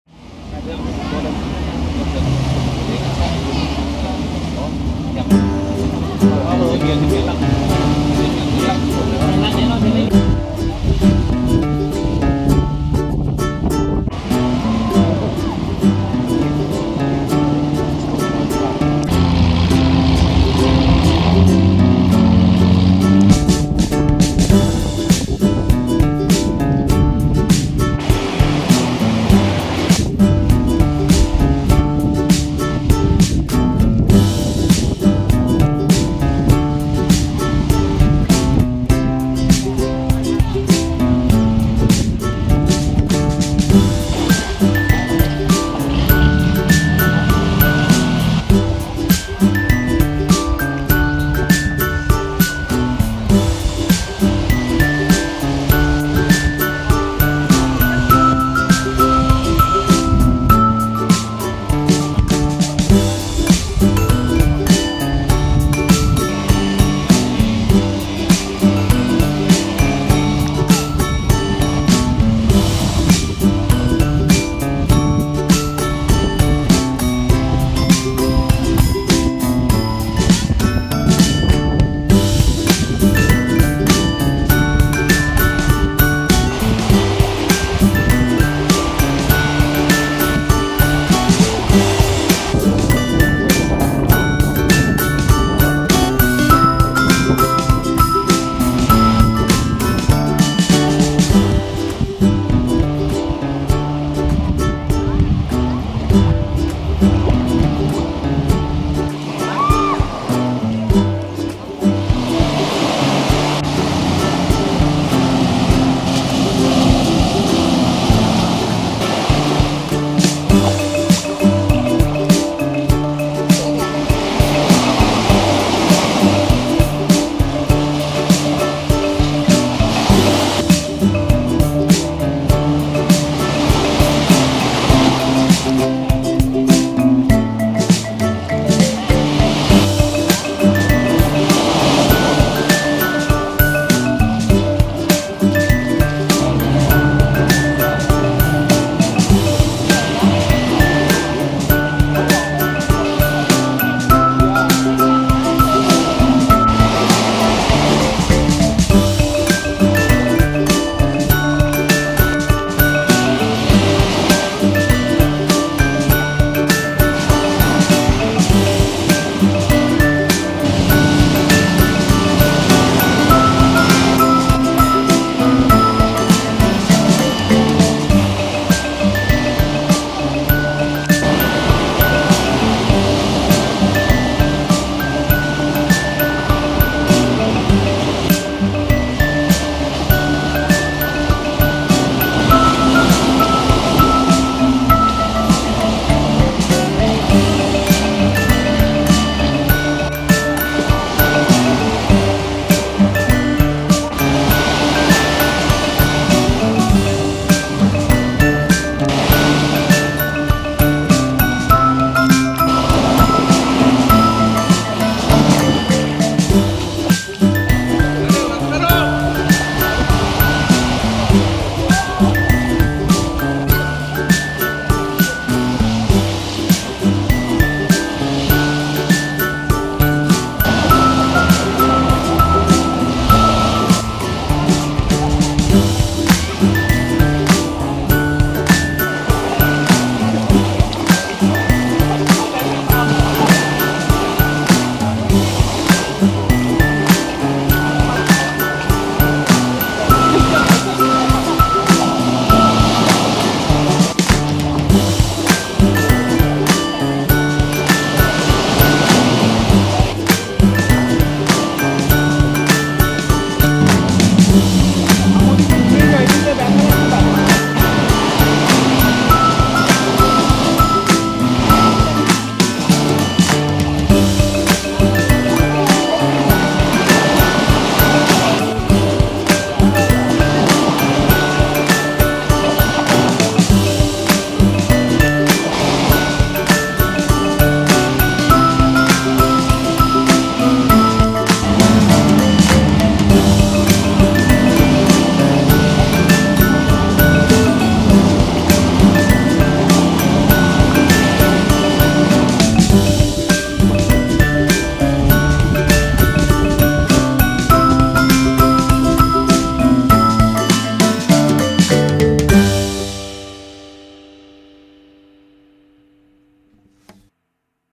Phra Nang Beach Railay Krabi Thailand
Phra Nang Beach is one of the best beaches in Krabi and one of the most beautiful places in Thailand. White sand, hanging  rocks attracts thousands of tourists from all over the world.

Пляж Прананг (Phra Nang Beach) – один из лучших пляжей Краби и одно из самых красивых мест Таиланда. Белый песок, нависающие над пляжем скалы создают этому месту неповторимый облик, который притягивает сюда тысячи туристов со всего мира.